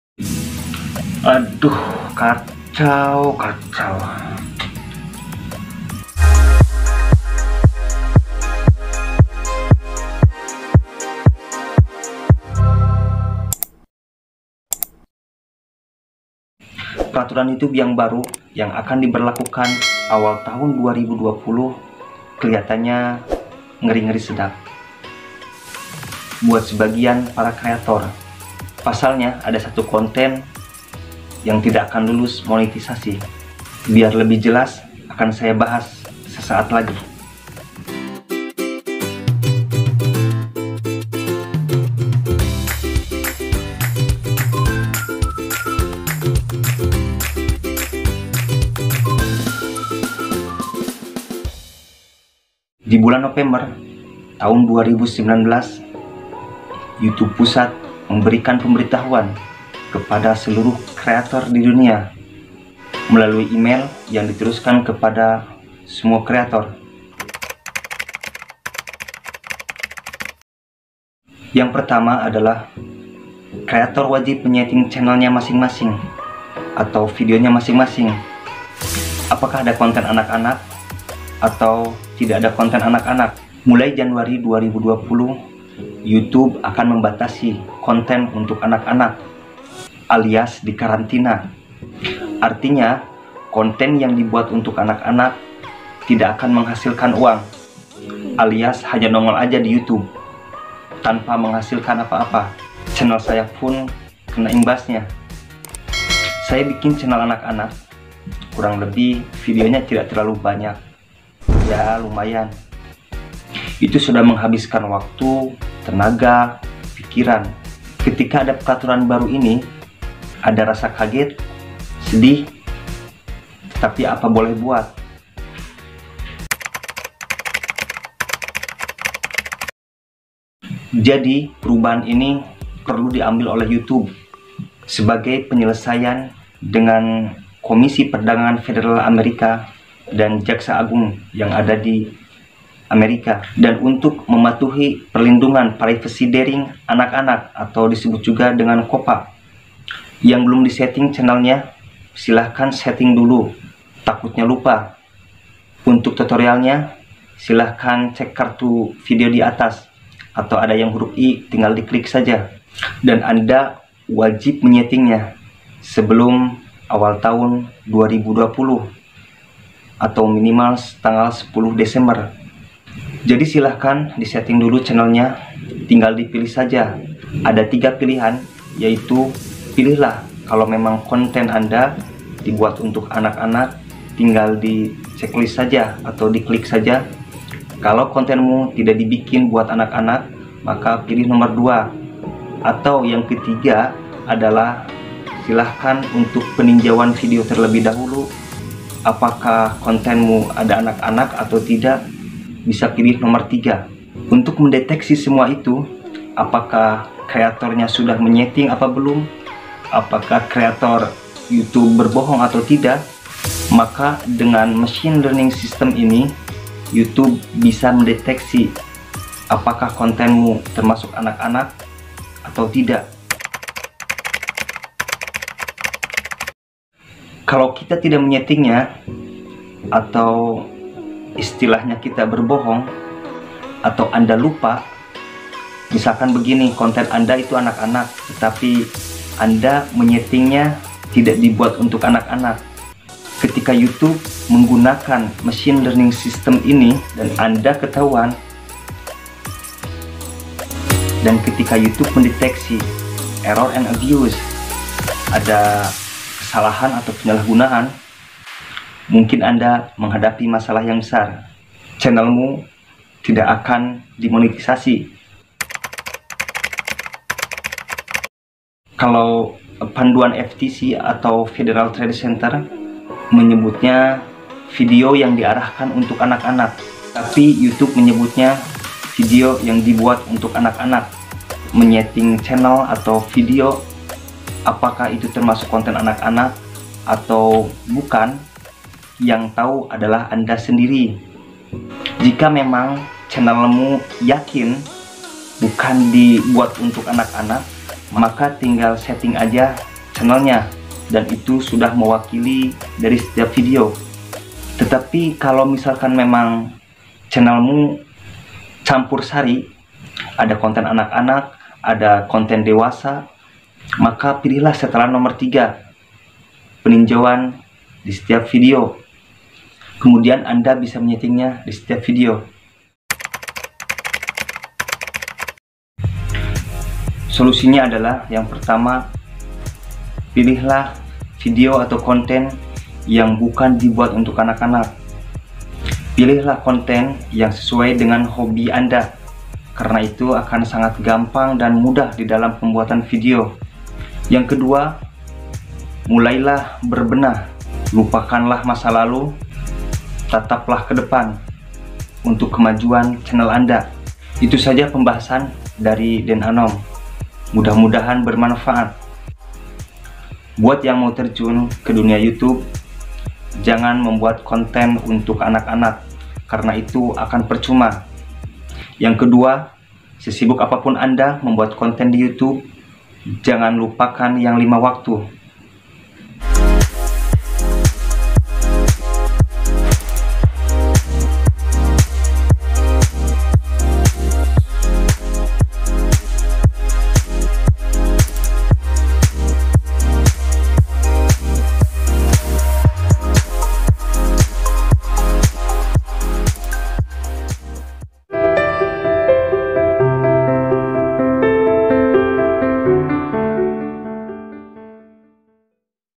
🔴Peraturan baru Youtube‼️ konten anak tidak akan di MONETISASI
mulai 2020 YouTube akan memberlakukan aturan baru ,mulai sekarang berbenah lah channel anda karena "KONTEN ANAK-ANAK" "KONTEN ANAK2" KONTEN ANAK"tidak akan di MONETISASI alias tidak akan menghasilkan uang/iklan.
✓tonton videonya biar ga gagal paham👌